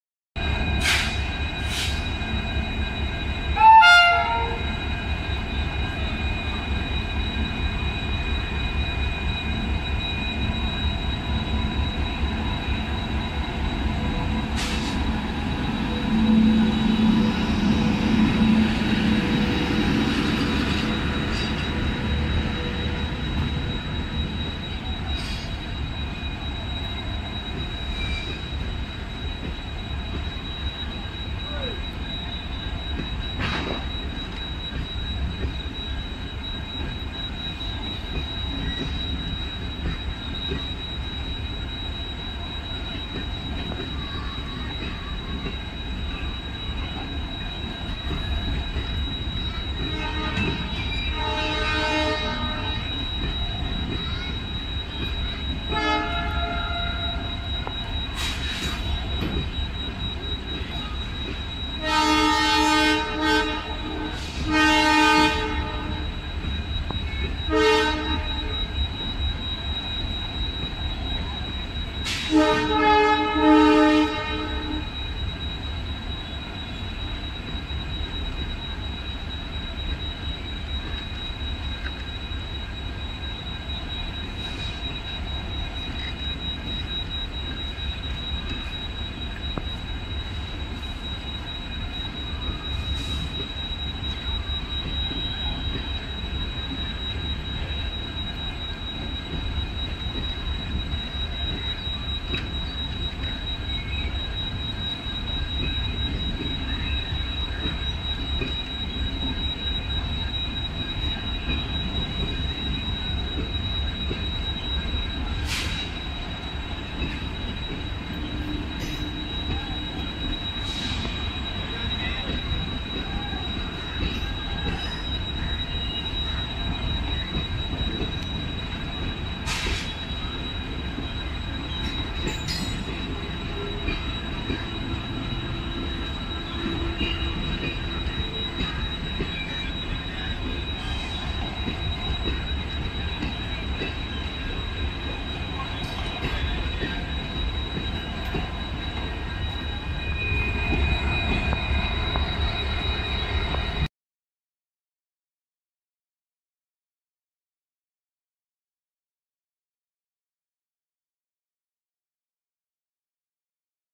16024 Malgudi SF Express
Malgudi SF Express train departs Yelahanka to Mysuru railway station hauled by WDP4D diesel locomotive with ICF coaches by South Western Railways, Indian Railways.

Captured on November 2019